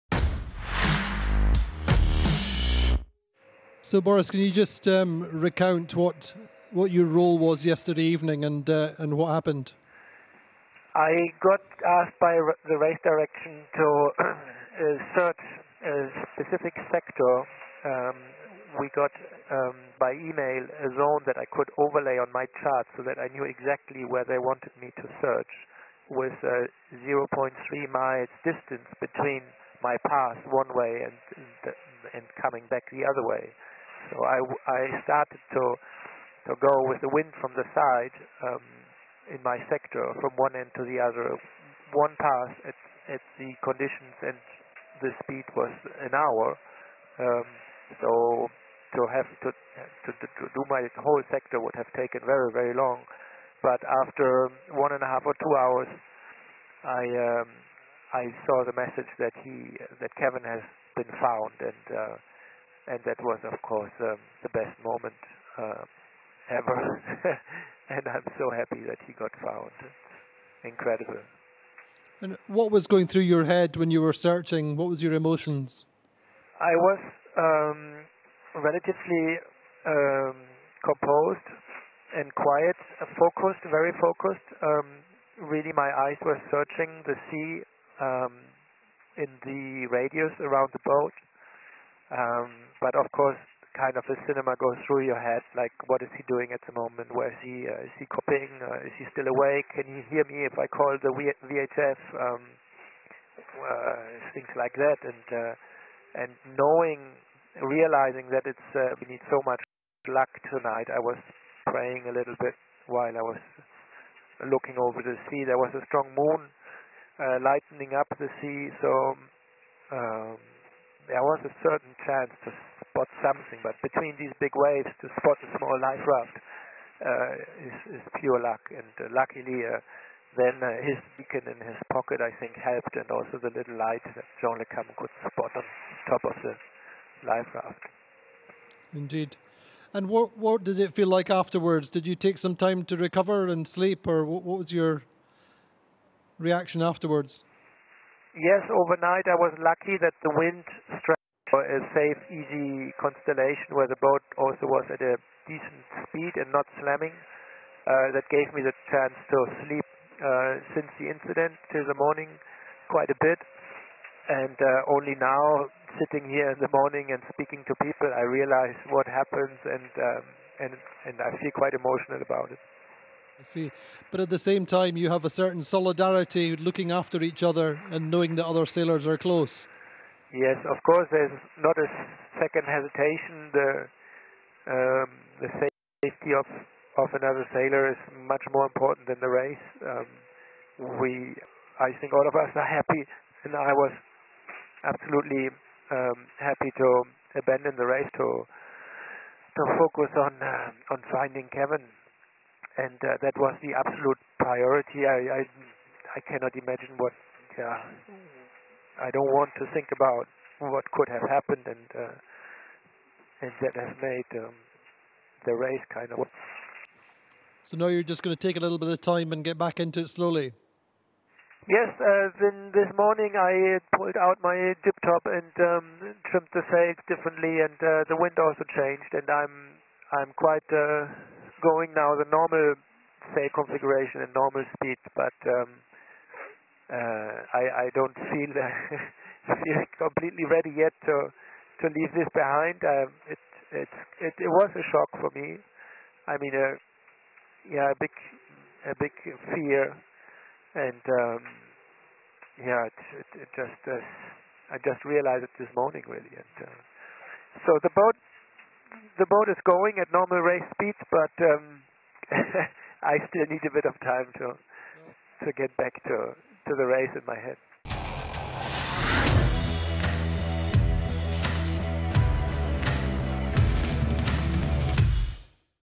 Day 23 - Boris explains about the search for Kevin Escoffier
"I was very focused on my task and had a search sector that I had to search very closely - with 600 metres between passages. One passage took me an hour. So it would have taken 15 to 20 hours to search the entire search field that was assigned to me. At this moment, of course, one realises how large the search field is and that one is looking for a needle in a haystack. I was very motivated and looked in all the right places as best I could. At the same time it was of course foreseeable that you would not manage to look for 10 hours. It was extremely cold with high swell and spray coming over the deck. There are limits somewhere. But it was great to see that the regatta management diverted all the boats that were available nearby and that we were able to search together with a fleet. So we gave luck every chance and luck finally led to Kevin being found. This is a great relief and now the tension is dropping. Of course there is also a certain sadness and the thoughts are with him. I am already back to my normal race, but I am not yet back to where the race is with my head, but that will certainly come soon. In any case, I will give myself some time today to let things sink in."